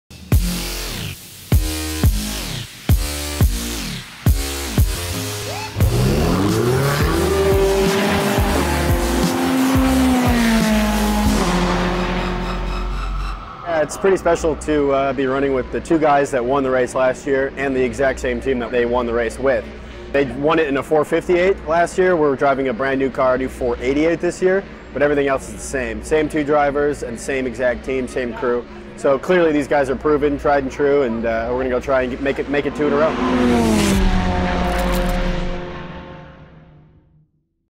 This Year's Team: 2017 Le Mans Cooper MacNeil Interview
Cooper MacNeil of WeatherTech Racing shares why he is excited to join the winning Scuderia Corsa team this year with drivers Bill Sweedler and Townsend Bell for the 24 Hours of Le Mans.